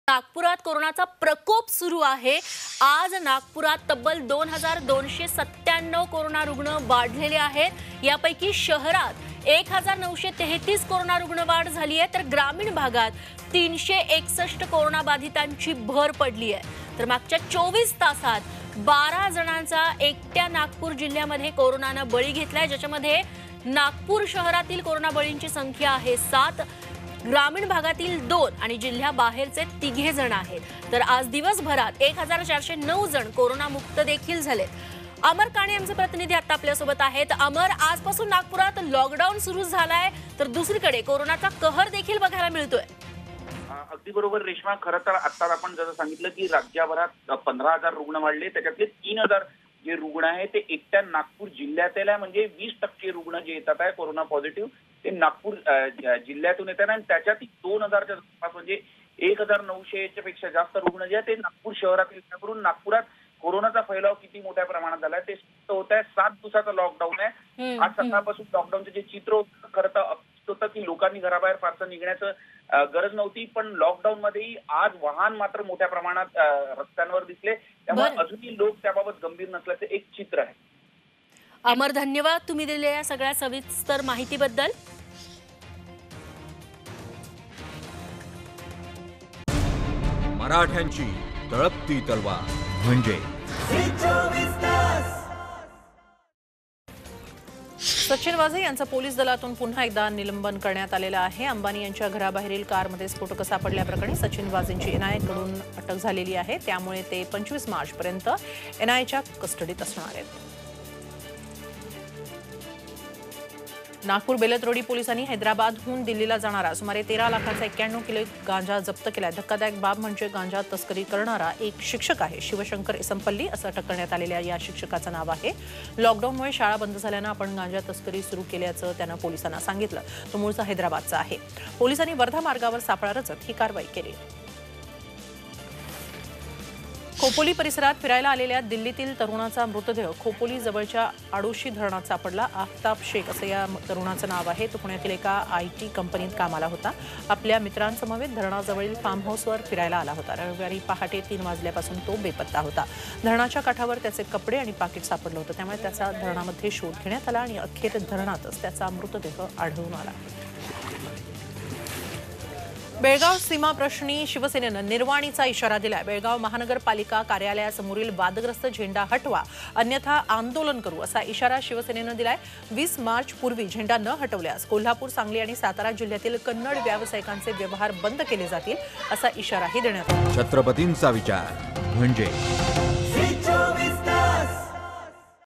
Lockdown असुनही 'या' शहरांत कोरोनाने गाठला उच्चांक
Nagpur 2297 Corona Patients Found Today